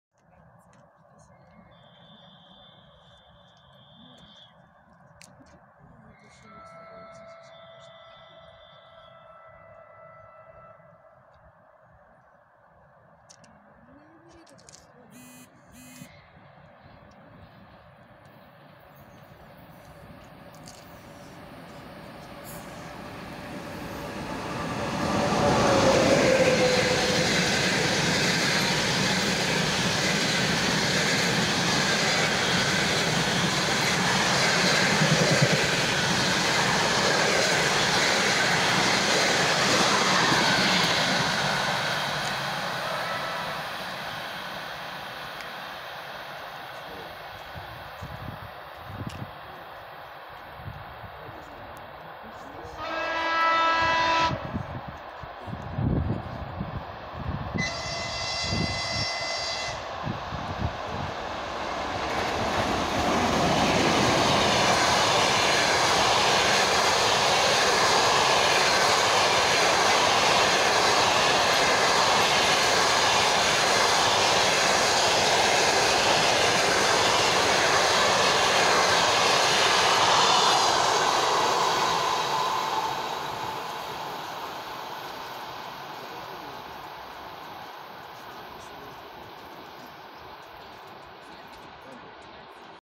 "мегадвиж" Чс4-055 с 118 Черновцы-Киев и Чс4-136 с 73 Москва-Львов пт.Тарасовка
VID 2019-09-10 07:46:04

чс4-055 с 118 Черновцы-Киев и Чс4-136 с 73 Москва-Львов(ВБС Москва-Хмельницкий) пл.Тарасовка